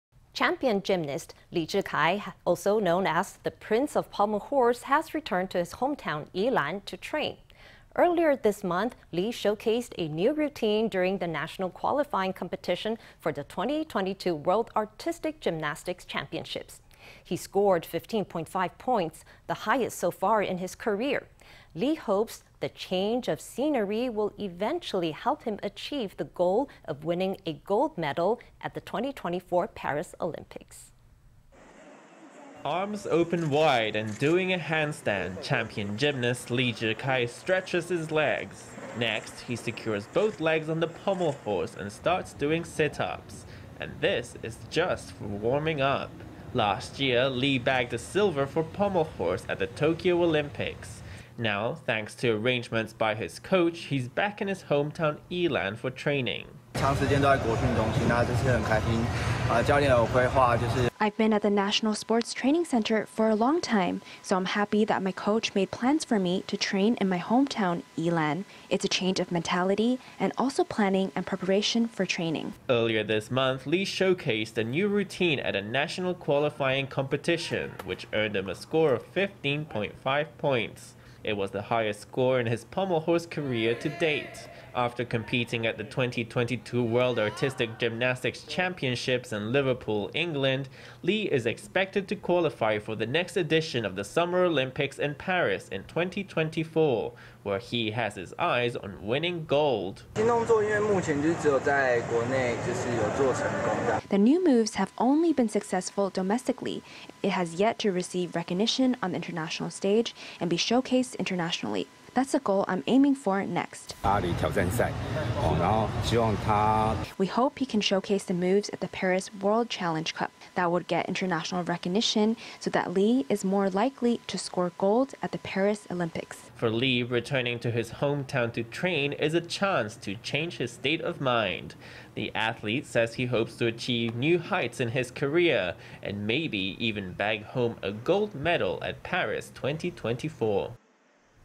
Pommel horse gymnast readying new moves in hopes of winning
Champion gymnast Lee Chih-kai, also known as “the prince of pommel horse”, has returned to his hometown, Yilan, to train. Earlier this month, Lee showcased a new routine during the national qualifying competition for the 2022 World Artistic Gymnastics Championships.  He scored 15.5 points, the highest so far in his career. Lee hopes the change of scenery will eventually help him achieve the goal of winning a gold medal at the 2024 Paris Olympics.

Arms open wide and doing a handstand, champion gymnast Lee Chih-kai stretches his legs. Next, he secures both legs on the pommel horse and starts doing sit-ups. And this is just for warming up. Last year, Lee bagged a silver for pommel horse at the Tokyo Olympics. Now, thanks to arrangements by his coach, he’s back in his hometown, Yilan, for training.

Lee Chih-kai
Gymnast
I’ve been at the National Sports Training Center for a long time, so I’m happy that my coach made plans for me to train in my hometown Yilan. It’s a change of mentality, and also planning and preparation for training.

Earlier this month, Lee showcased a new routine at a national qualifying competition, which earned him a score of 15.5 points. It was the highest score in his pommel horse career to date. After competing at the 2022 World Artistic Gymnastics Championships in Liverpool, England, Lee is expected to qualify for the next edition of the Summer Olympics in Paris in 2024, where he has his eyes on winning gold.

Lee Chih-kai
Gymnast
The new moves have only been successful domestically. It has yet to receive recognition on the international stage and be showcased internationally. That’s the goal I’m aiming for next.

Lin Yu-hsin
Coach
We hope he can showcase the moves at the Paris World Challenge Cup. That would  get international recognition, so that Lee is more likely to score gold at the Paris Olympics.

For Lee, returning to his hometown to train is a chance to change his state of mind. The athlete says he hopes to achieve new heights in his career and maybe even bag home a gold medal at Paris 2024.